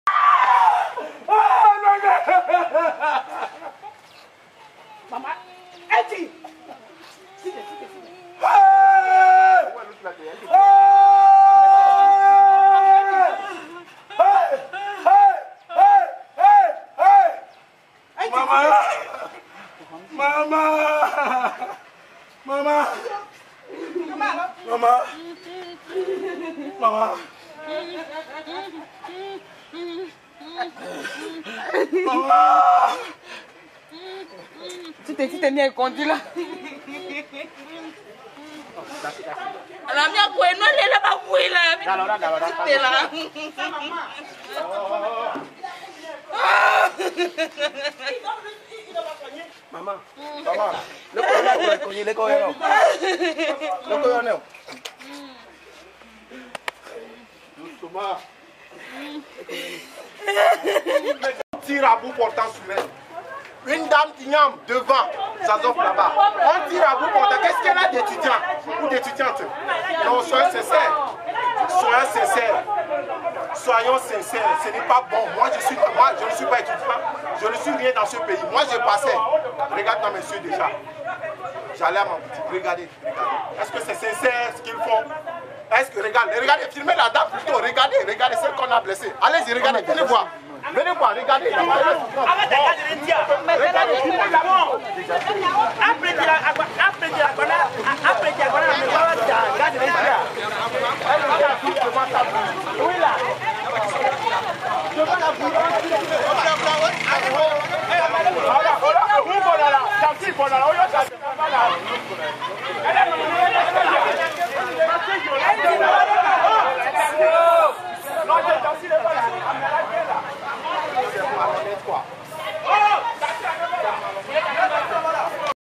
Etat de terreur au Togo: une dame victime d'un tir à bout portant des gendarmes [1er Juillet 2011]
Une dame est victime d'un tir à bout portant pendant qu'une manifestation des étudiants de l'Université de Lomé est dispersée sauvagement par les gendarmes. La violence est la seule réponse du régime de Faure Gnassingbé aux problèmes sociaux dans un pays où en l'absence de services de secours il faut transporter les victimes à moto pour l'hôpital.